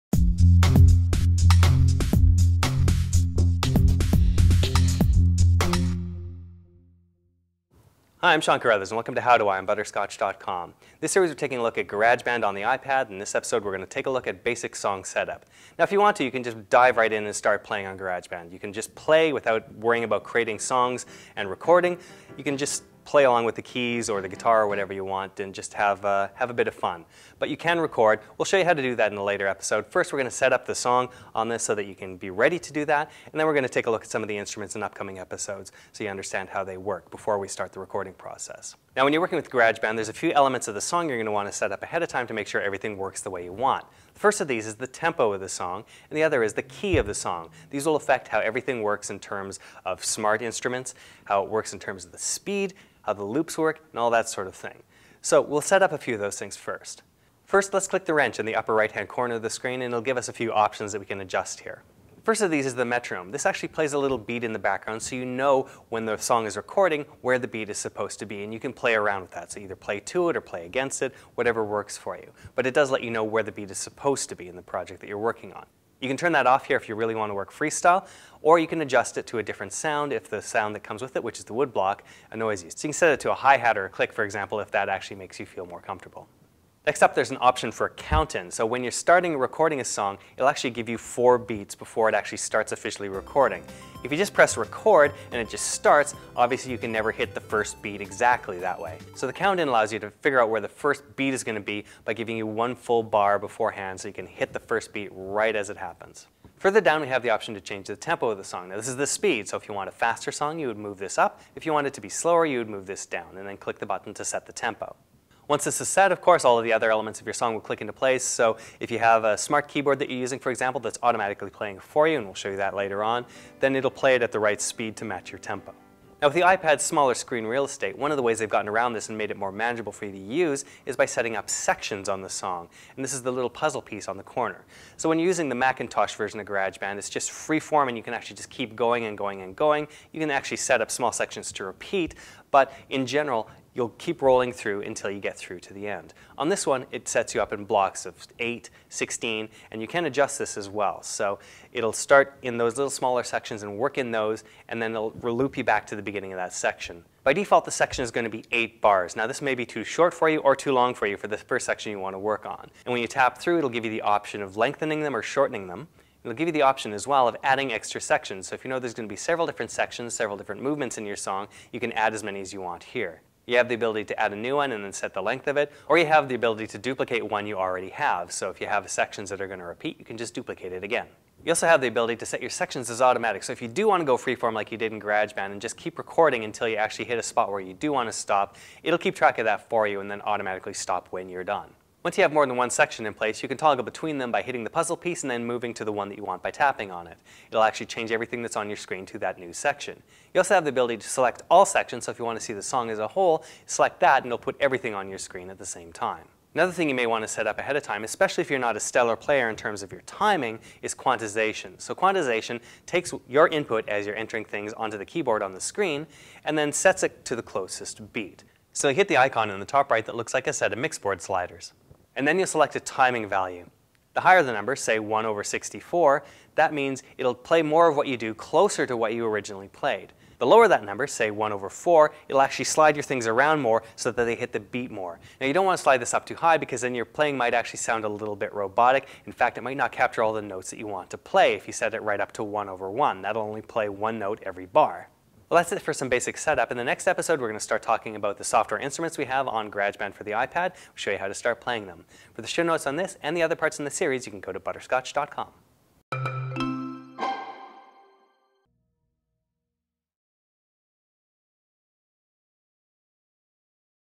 Song settings in GarageBand
Before creating a song using the GarageBand iPad app you might want to adjust some of the internal song settings. These control the tempo and key of your song as well as the metronome and count in time. We'll show you how to do that and more in this episode @ butterscotch.com